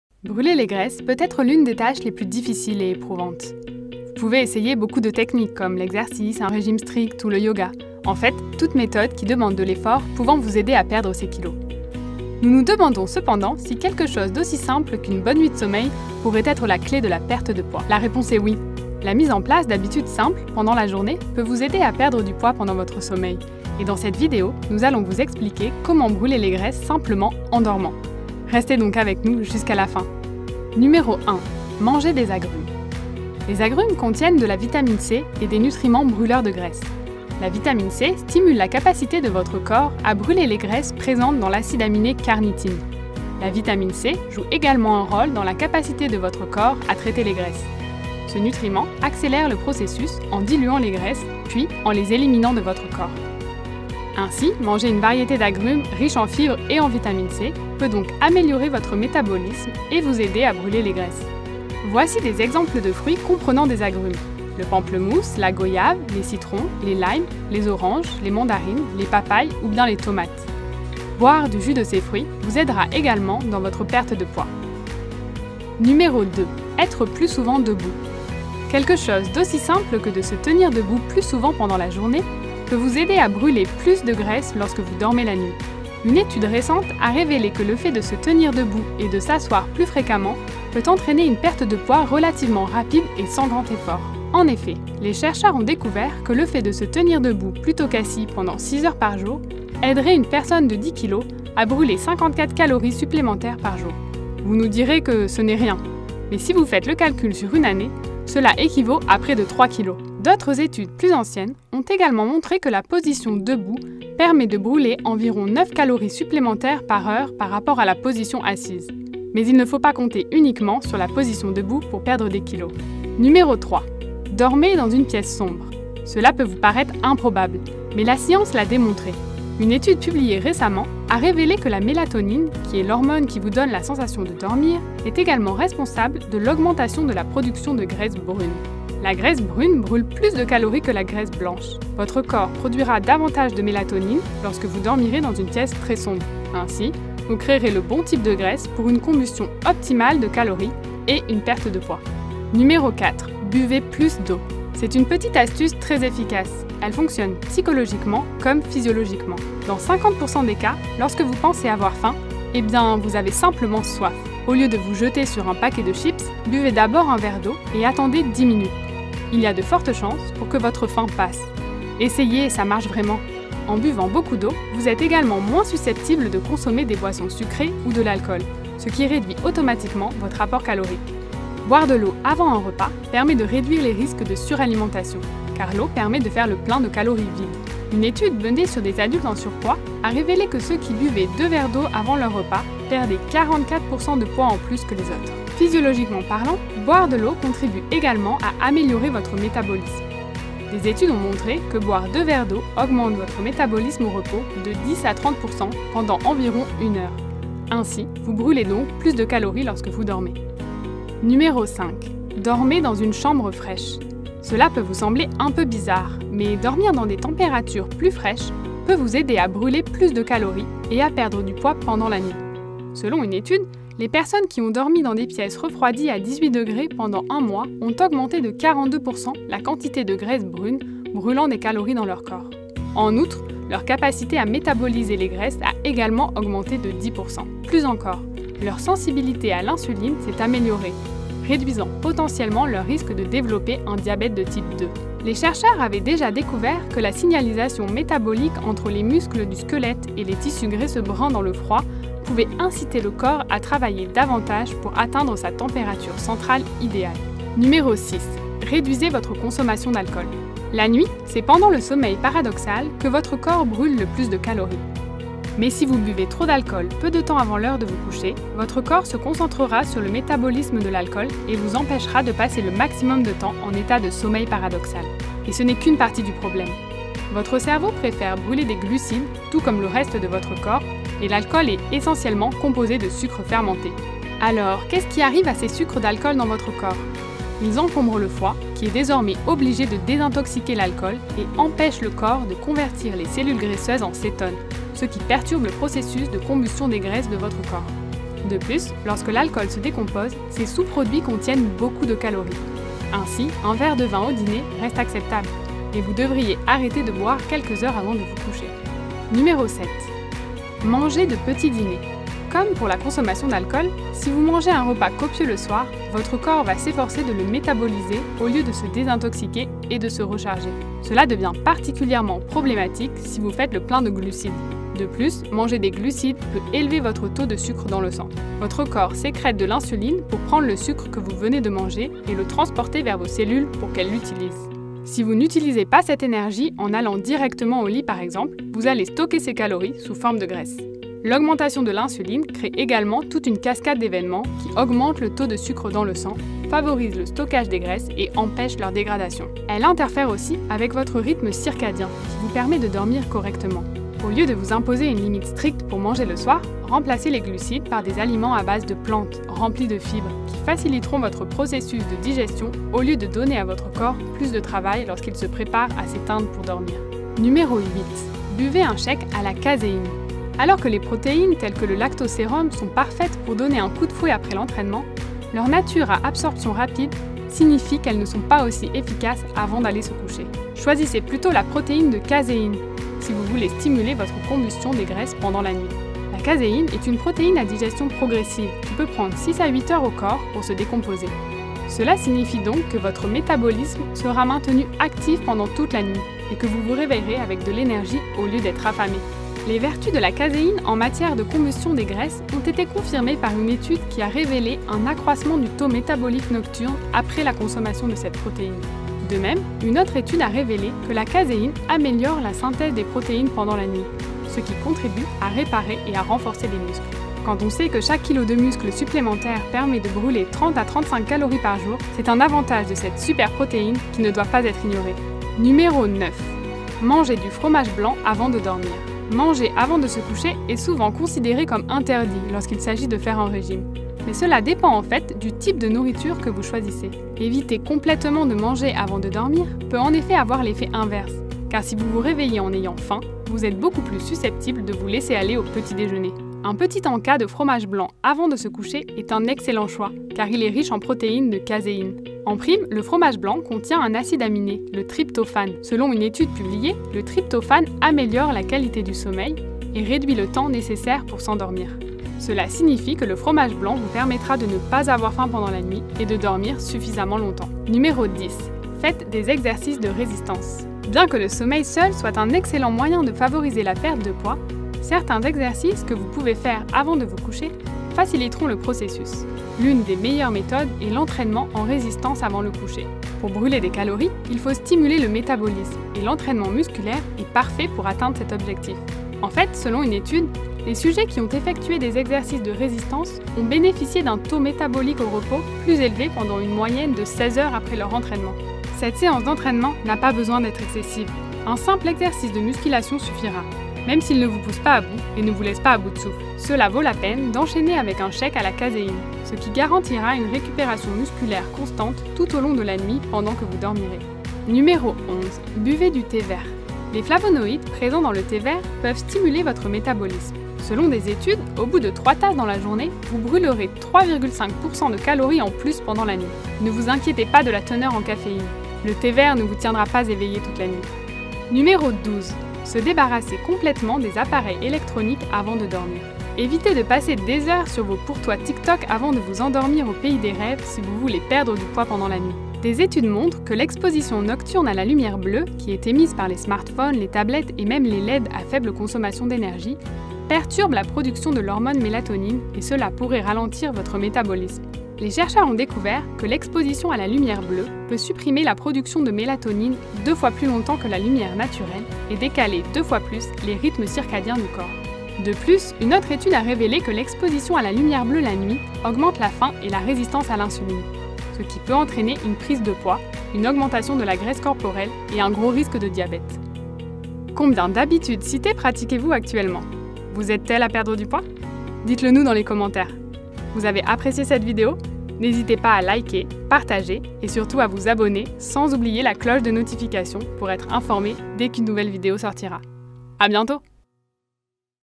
Perdre Du Poids En Dormant, C'est Possible !
😴 Perdre Du Poids En Dormant ? 😴 C'est Possible avec Des Habitudes Faciles à Mettre En Place Avant De Se Coucher !

À PROPOS DE LA VIDÉO

Beaucoup de femmes se demandent s'il est possible de brûler des graisses et de perdre du poids pendant leur sommeil.

Ainsi, nous nous demandons si quelque chose d'aussi simple qu'une bonne nuit de sommeil pourrait être la clé de la perte de poids ?

La réponse est oui !  La mise en place d'habitudes simples pendant la journée peut vous aider à perdre du poids pendant votre sommeil et dans cette vidéo, nous allons vous expliquer comment brûler des graisses simplement en dormant.

1. Mangez des agrumes
Les agrumes contiennent de la vitamine C, celle-ci peut améliorer votre métabolisme et vous aider à brûler les graisses.

2. Être plus souvent debout
Se tenir debout et s'asseoir plus fréquemment peut entraîner une perte de poids relativement rapide et sans grand effort.

3. Dormez dans une pièce sombre
La mélatonine, qui est l'hormone qui vous donne la sensation de dormir, est également responsable de l'augmentation de la production de graisse brune qui permet une combustion optimale de calories et une perte de poids.

4. Buvez plus d'eau
Boire deux verres d'eau augmente votre métabolisme au repos de 10 à 30 % pendant environ une heure. Ainsi, vous brûlez donc plus de calories lorsque vous dormez.

5. Dormez dans une chambre fraîche
Dormir dans des températures plus fraîches peut vous aider à brûler plus de calories et à perdre du poids pendant la nuit.

6. Réduisez votre consommation d'alcool
C'est pendant le sommeil paradoxal que votre corps brûle le plus de calories. Si vous buvez trop d'alcool, votre corps se concentrera sur le métabolisme de l'alcool et vous empêchera de passer le maximum de temps en état de sommeil paradoxal.

7. Mangez de petits dîners
si vous mangez un repas copieux le soir, votre corps va s'efforcer de le métaboliser au lieu de se désintoxiquer et de se recharger.
 8. Buvez un shake à la caséine
La caséine peut prendre six à huit heures au corps pour se décomposer. Cela signifie donc que votre métabolisme sera maintenu actif pendant toute la nuit.
9. Mangez du fromage blanc avant de dormir
En prime, le fromage blanc contient un acide aminé, le tryptophane.Le tryptophane améliore la qualité du sommeil et réduit le temps nécessaire pour s'endormir.

10. Faites des exercices de résistance
Pour brûler des calories, il faut stimuler le métabolisme, et l'entraînement musculaire est parfait pour atteindre cet objectif.

11. Buvez du thé vert
Au bout de trois tasses de thé dans la journée, vous brûlerez 3,5 % de calories en plus pendant la nuit.

12. Se débarrasser complètement des appareils électroniques avant de dormir
L'exposition nocturne à la lumière bleue, qui est émise par les smartphone, perturbe la production de l'hormone mélatonine et cela pourrait ralentir votre métabolisme.

⭐️⭐️ Toutes les semaines, découvrez 3 nouvelles vidéos avec FitZone ⭐️⭐️